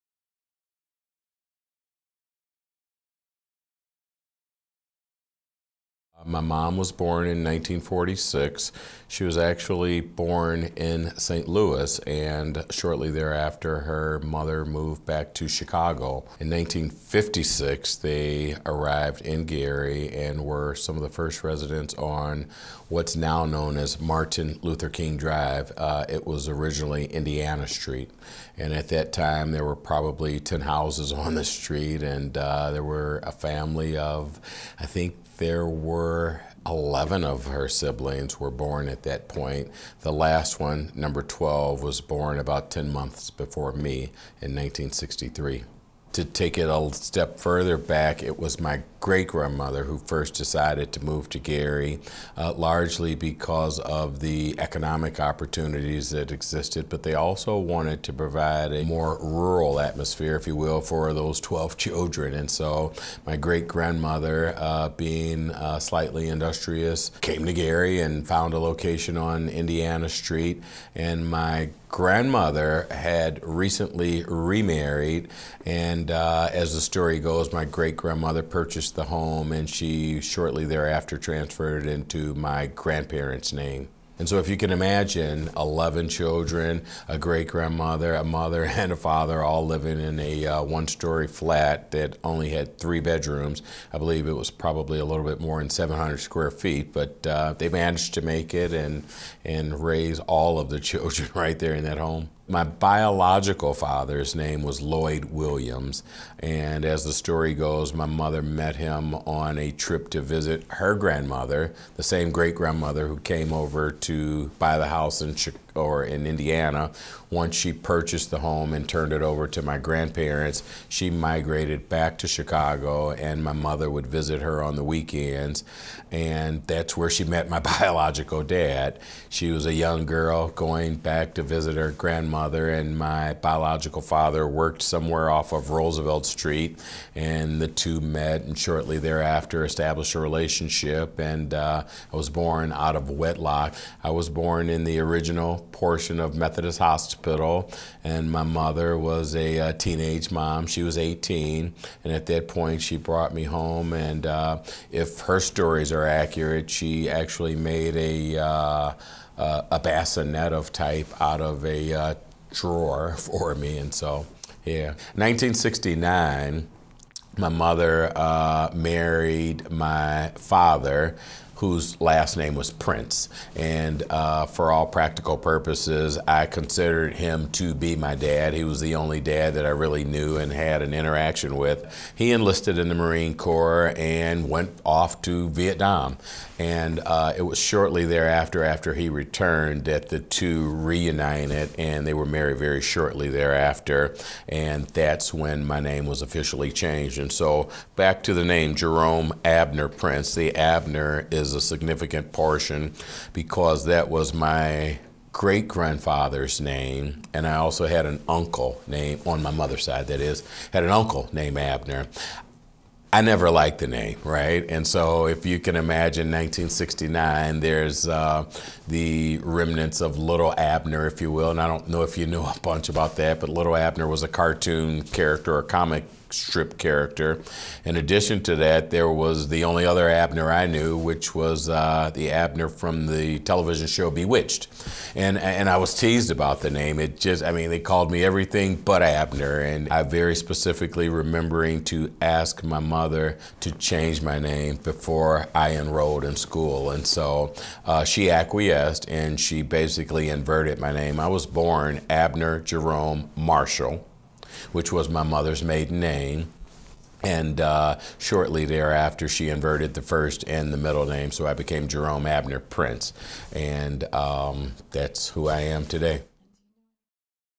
Part 1: That's Who I Am
“She actually made a bassinet out of a drawer for me.”

An oral history interview with Mayor Jerome Prince of Gary, Indiana.

~ a Welcome Project Flight Paths story ~

Find more at welcomeproject.valpo.edu or listen to our weekly podcast by searching for Welcome Project on your favorite app.